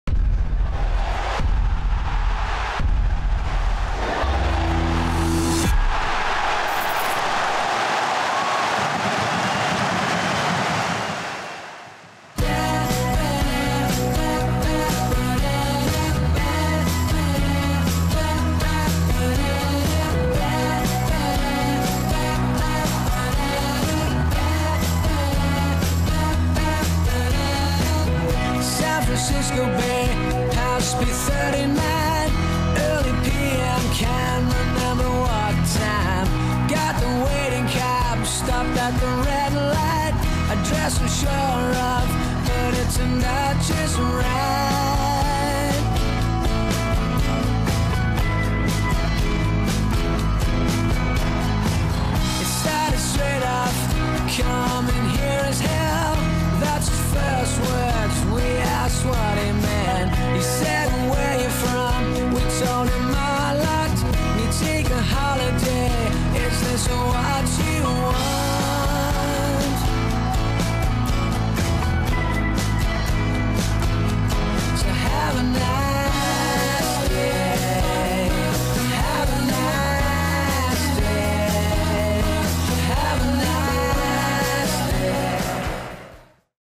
FLASHBACK | Royals overcome the Toon back in 2007
A flashback to a Premier League win over Newcastle United, with two memorable strikes from Dave Kitson and Shane Long at Madejski Stadium...